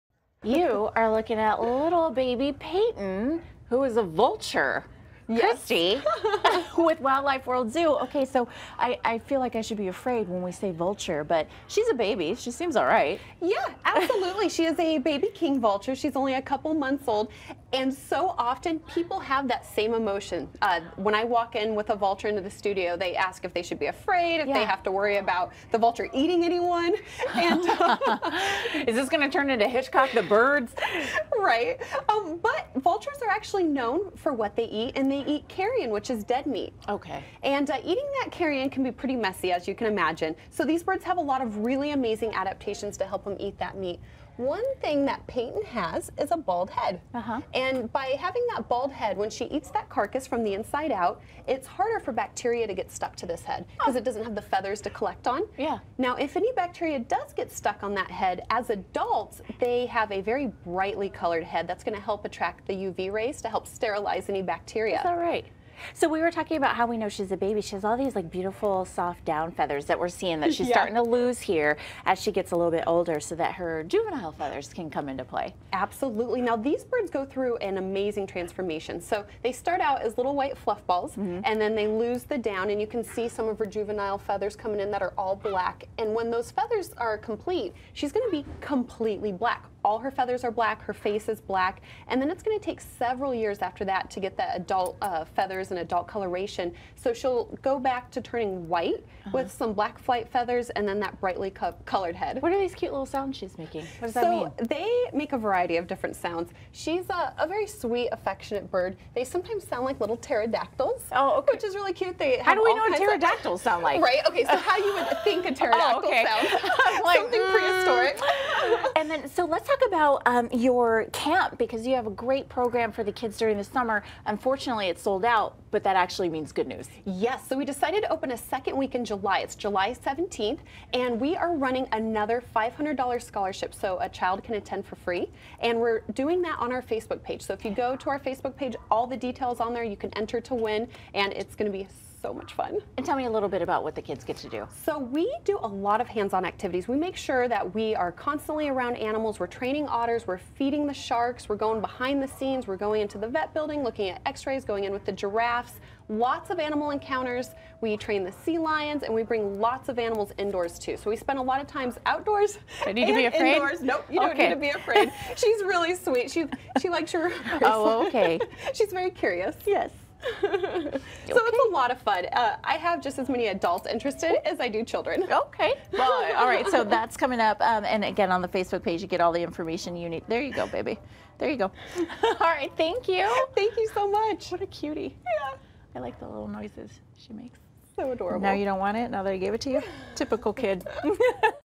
Wildlife World Zoo's baby vulture
Wildlife World Zoo stopped by the newsroom to introduce to their baby king vulture.&nbsp;